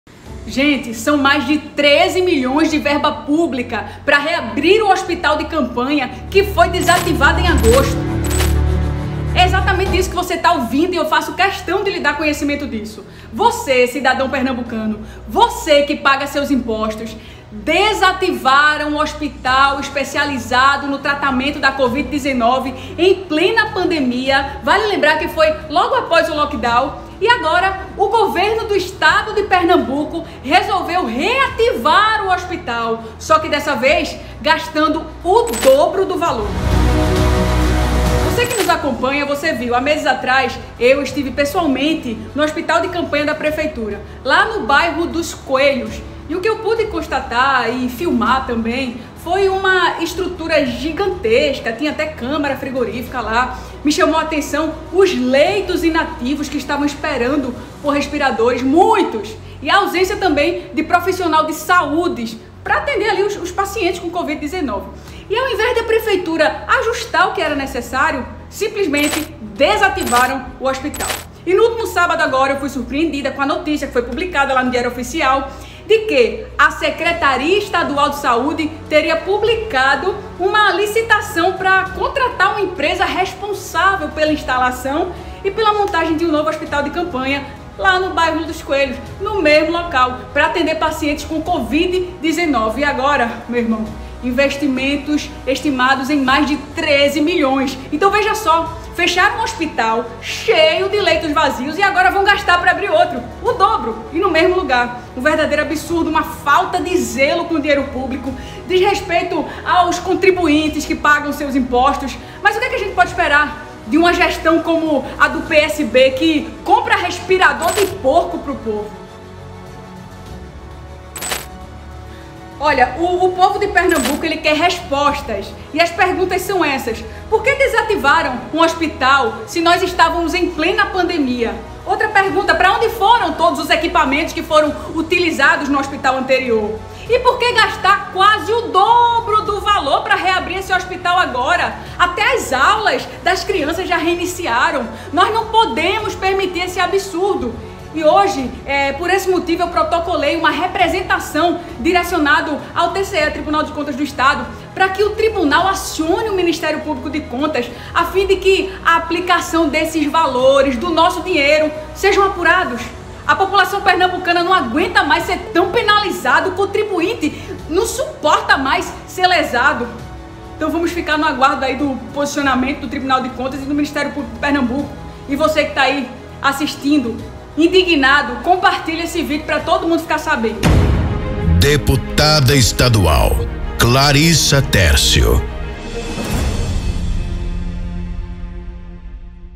Mais de 13 Milhões
Nesse vídeo, denuncio a pretensão do Governo do Estado de Pernambuco em gastar algo em torno de R$ 13,3 milhões para instalar e montar novo hospital de campanha, no bairro dos Coelhos, mesmo local onde funcionou o da Prefeitura do Recife, desativado em agosto.
Na época, a justificativa utilizada foi a queda do número de casos da Covid-19, para fechar o equipamento que custou aos cofres públicos R$ 7,5 milhões.

Ou seja, o Estado pretende gastar quase o dobro para contratar empresa responsável para instalar e montar toda estrutura, segundo aviso de licitação, publicado, por meio de Errata, da edição extra do Diário Oficial do último sábado.

Não podemos permitir esse absurdo. Por isso, protocolei representação junto ao Tribunal de Contas do Estado (TCE) para que fiscalize e acompanhe a aplicação desses recursos. O interesse público está sempre em primeiro lugar!